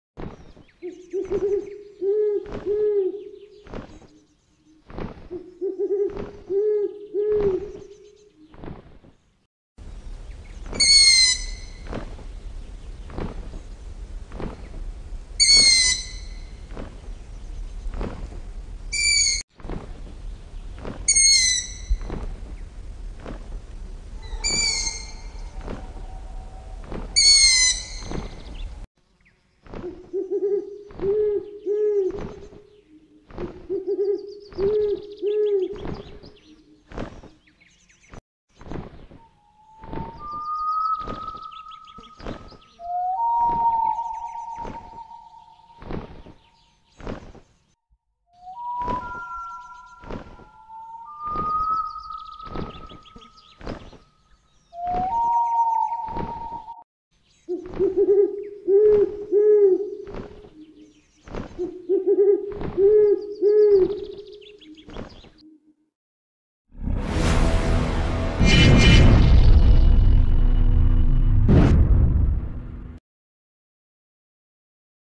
BIRD OWL animation on a green screen. The video is on the GREEN SCREEN.
Footage of an owl bird on a green screen for video editing. Chromakey animation of an owl from different angles for insertion into a video.

MORE ANIMALS

INSECTS

 MONSTERS